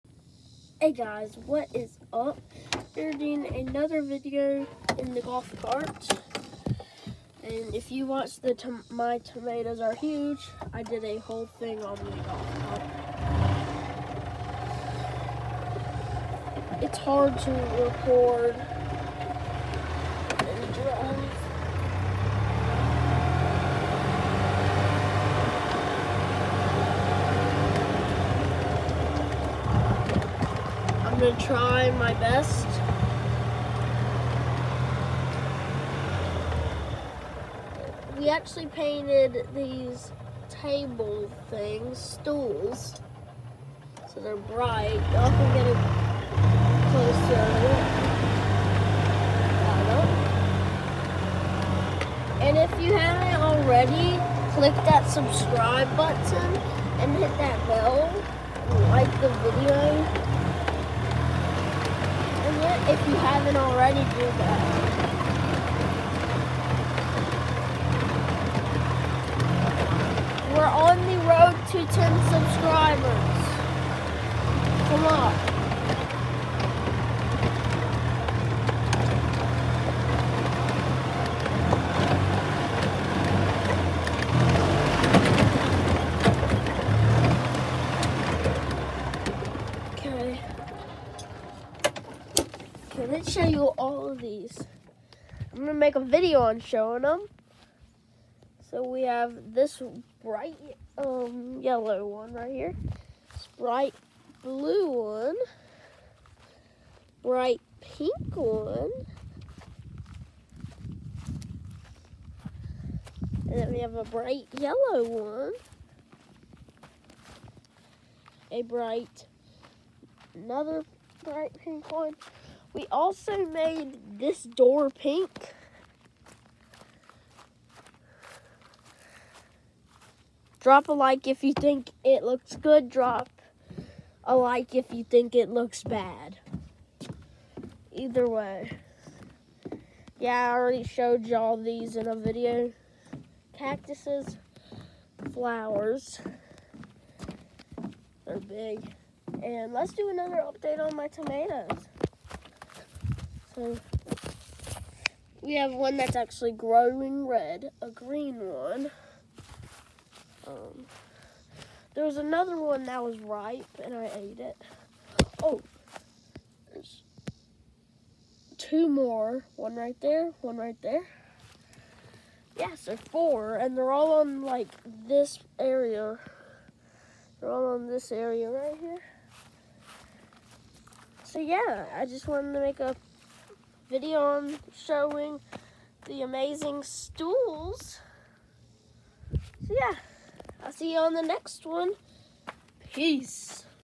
Showing colorful spools they are pretty
The stools my grandfather made are very pretty and my grandmother made on the paint in them and they’re beautiful